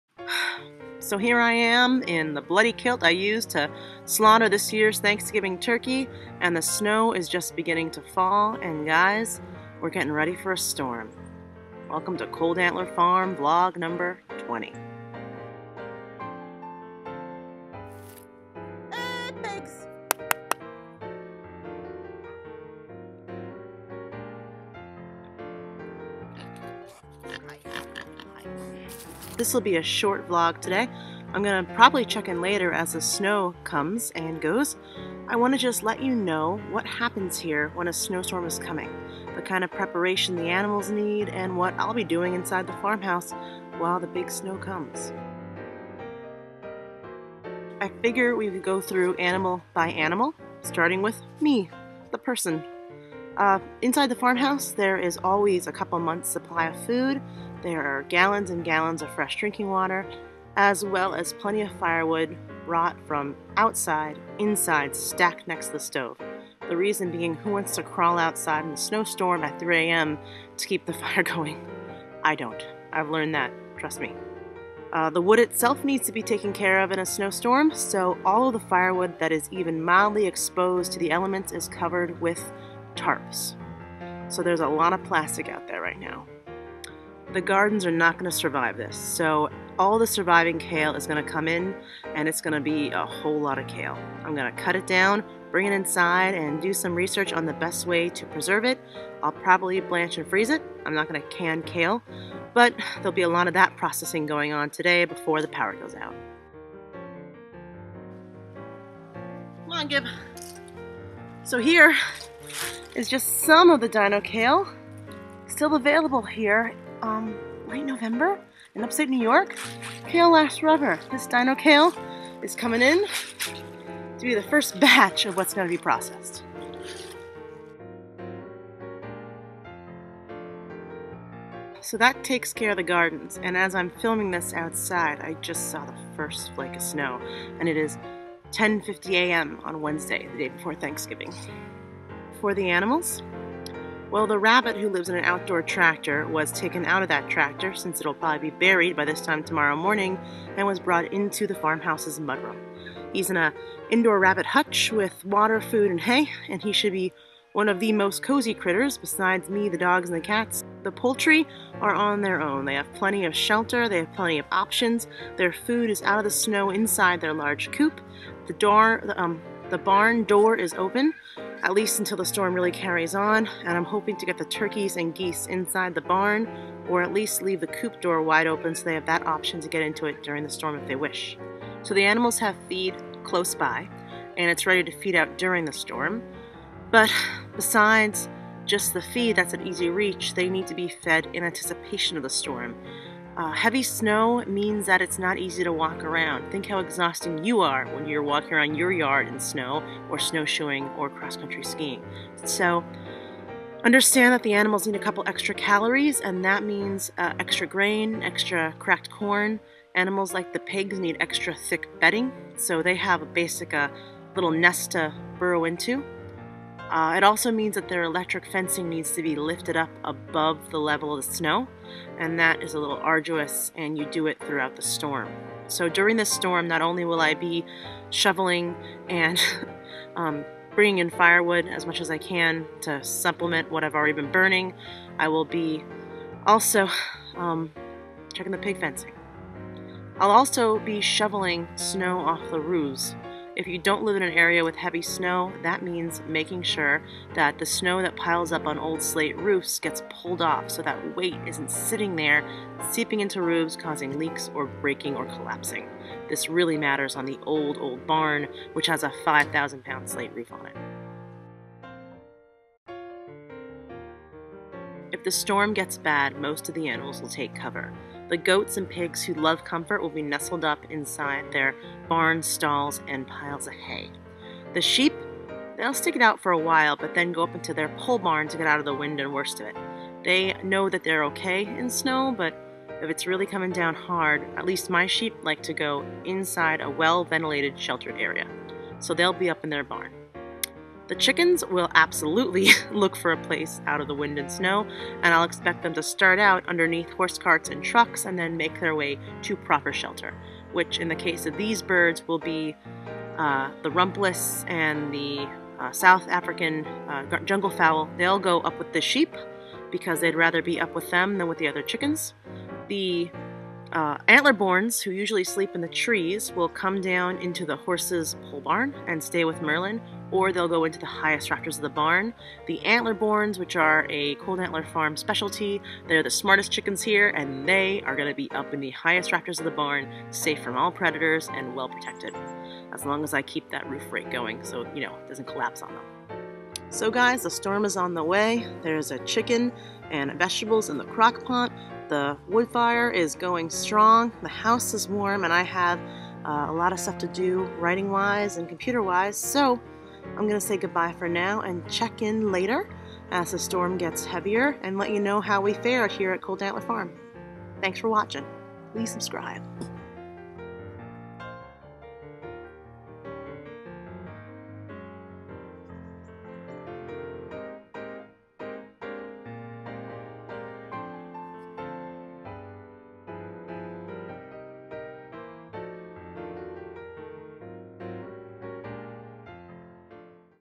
Episode 20: Snow Storm On the Way!
A storm is on the way to Cold Antler, and possibly a bad one. Here is a video filmed as the first flakes are falling and some (not all) of the prep that goes into getting a small homestead ready for the first big storm of the season! For more on the whole story go to barnheart.com